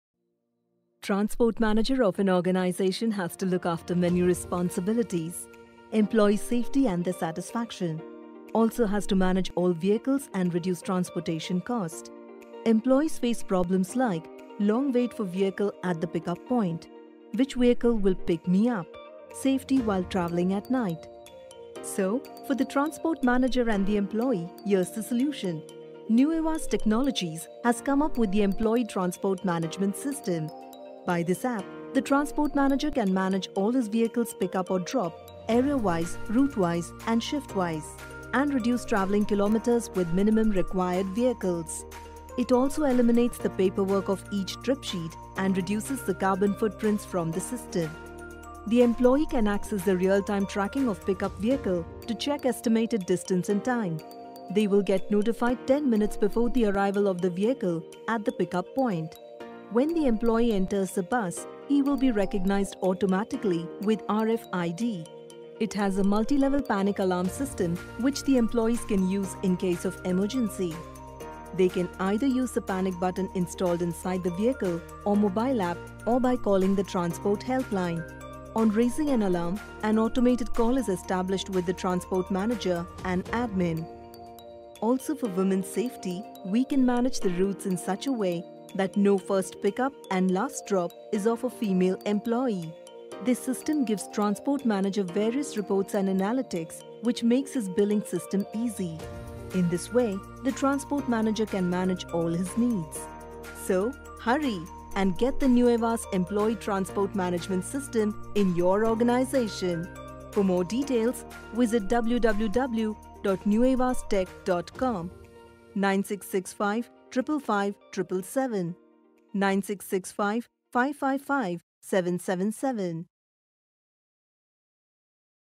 Animated video| animation services in pune|VideoNx
Check out some recent animatedvideo of nuevastechnology which is our client . The idea behind creating this animatedstylevideo was convince the audience that the nuevas app is very useful for office employee for travelling purpose and safe too.videoproductioncompanyinpune animationservicesinpune VideoNx.